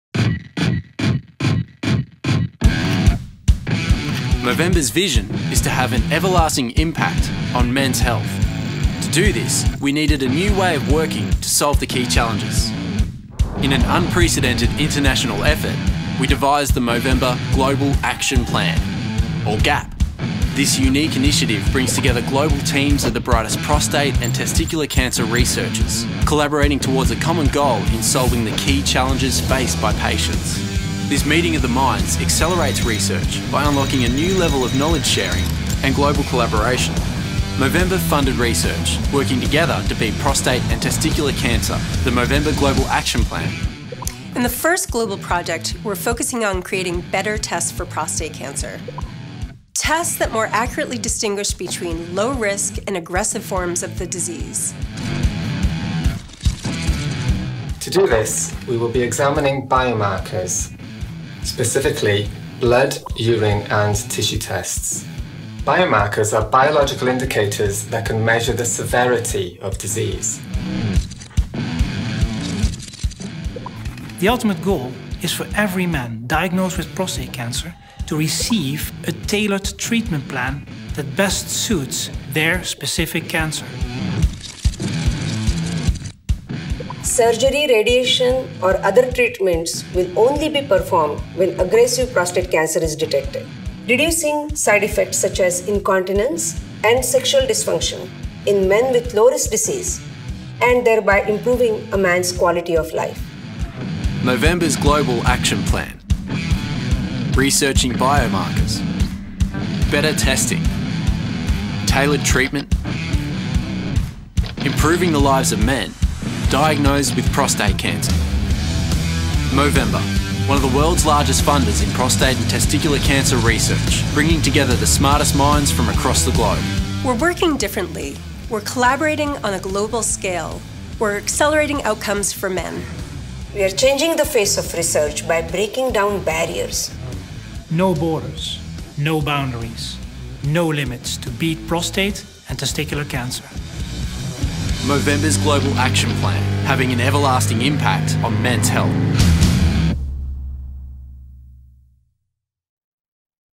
Movembers Global Action Plan: biomarkører (Norsk bokmål)
Movembers Global Action Plan samler globale team av de skarpeste forskerne på prostata- og testikkelkreft for å løse viktige utfordringer som pasienter og deres familier står overfor.

Takket være deg og pengene du samlet inn, har et nylig Global Action Plan forskningsprosjekt fokusert på å undersøke biomarkørersom blod, urin og vev, som kan måle alvorlighetsgrad for prostatakreft hos pasienter. Dette kan gi oss bedre tester for å diagnostisere prostatakreft. Movember og våre støttespillere forandrer menns helse.